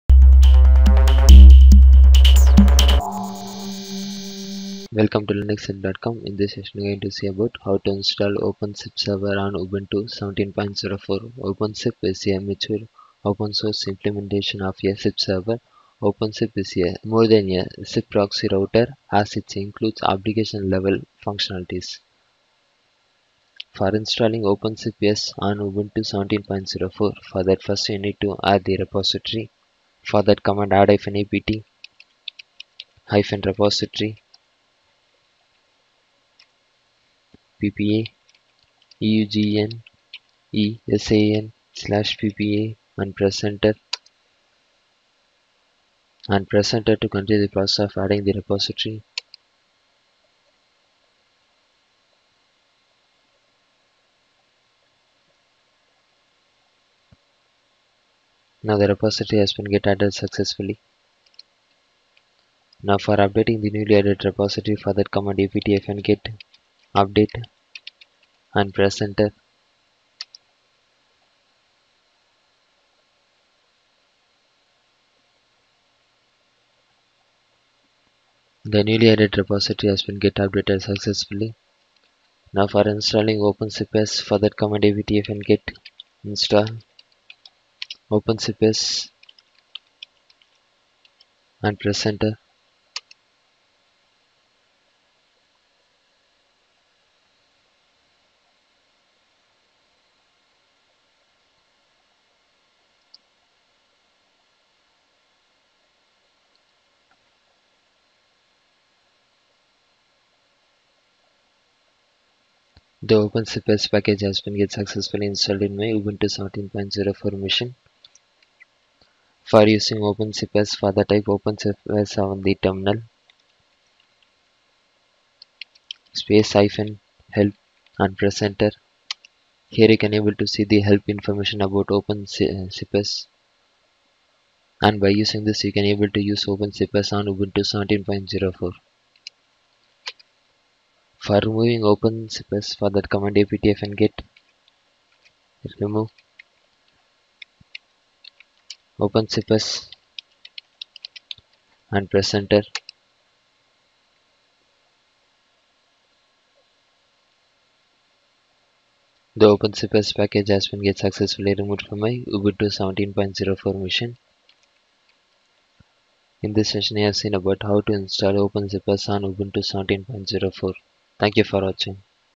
How to install OpenSIPS on Ubuntu 17.04
This video covers the installation of OpenSIPS on Ubuntu. OpenSIPS is a free software implementation of the session initiation protocol (SIP) for voice over IP (VoIP).